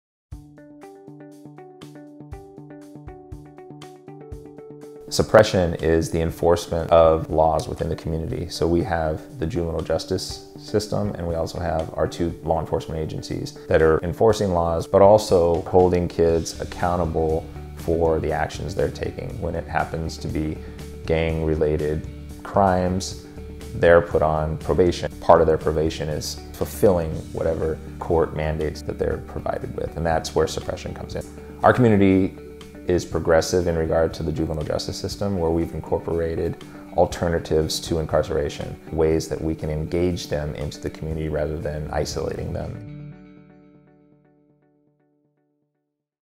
Suppression; Five Core Strategies
The OJJDP Gang Prevention and Intervention model refers to suppression as involving both formal and informal social control procedures, including close supervision or monitoring of gang youth by agencies of the criminal justice system and also by community-based agencies, schools, and grassroots groups.